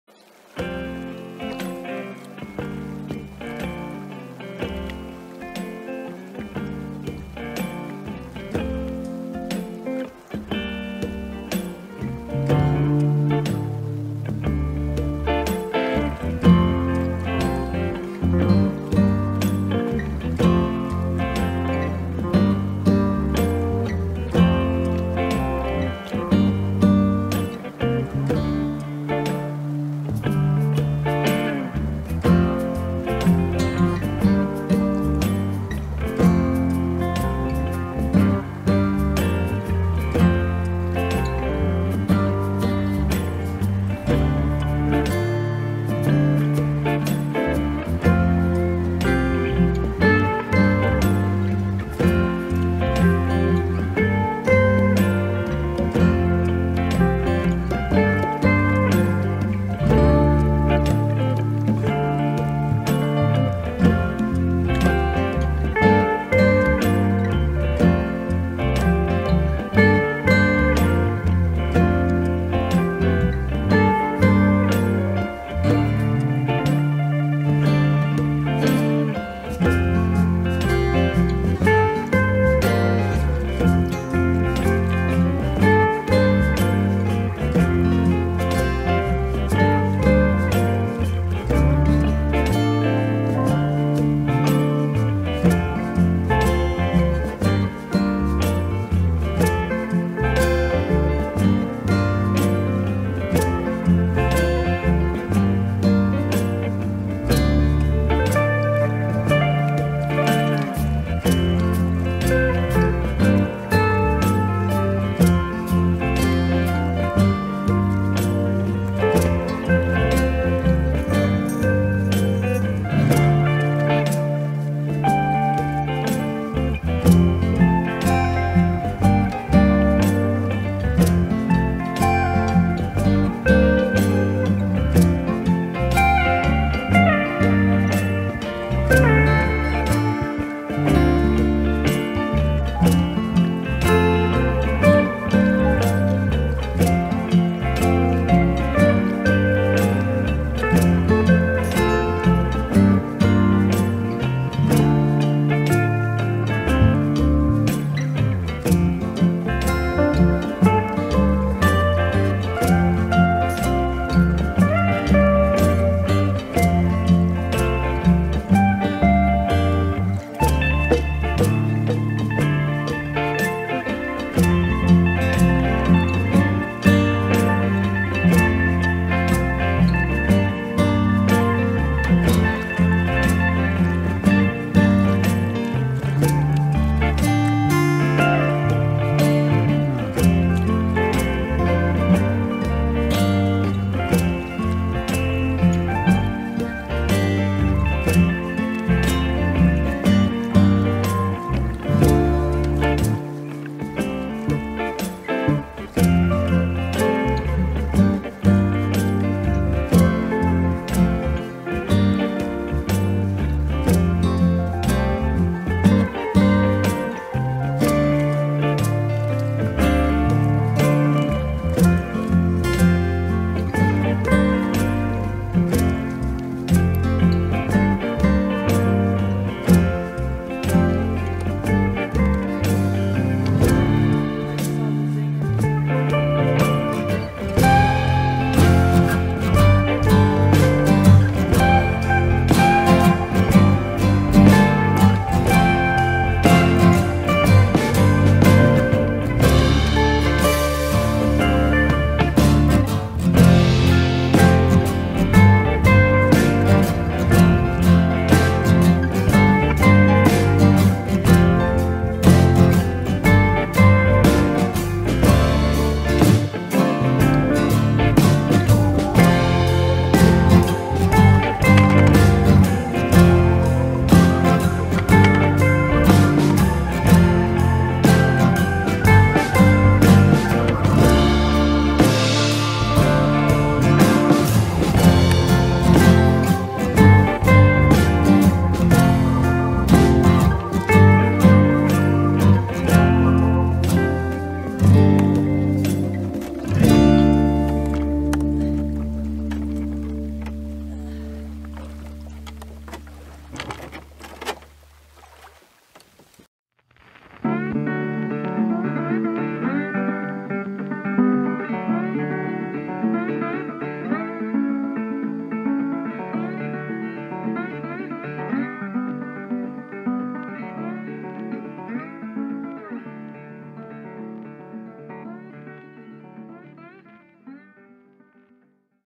Enjoying the Last Few Minutes of Kayaking
Yep, waiting for our turn at the boat ramp. It was time to get out of the water.